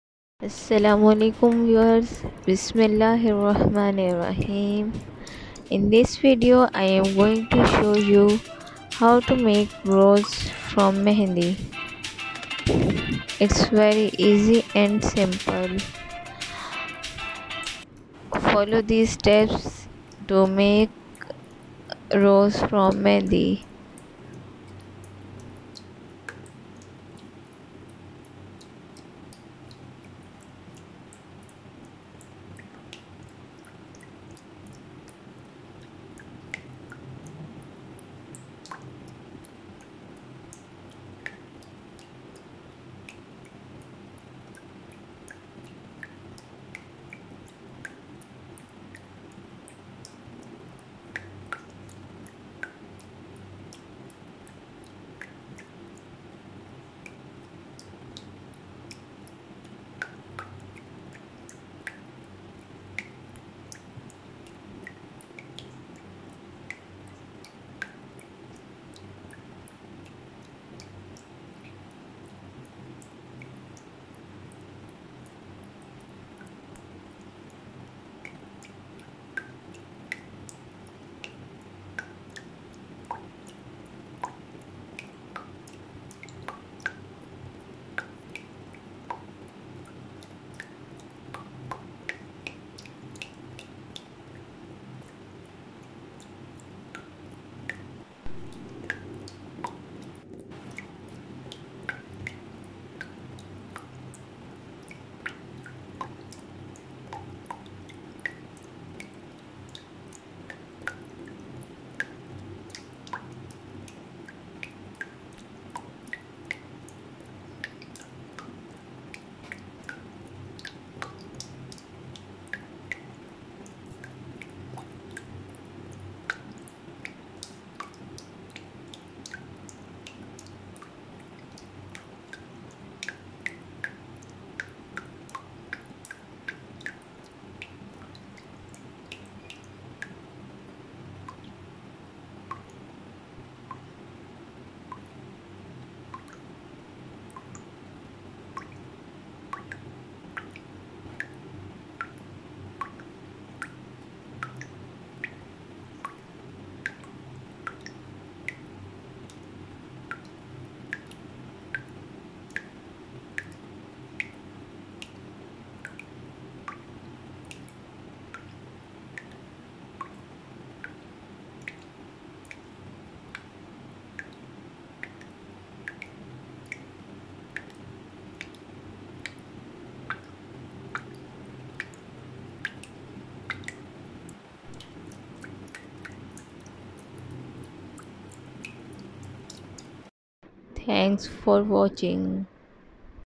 How to make rose with mehndi | Arabic rose design | Easy design for beginners
In this video i have shown how to make rose with mehndi.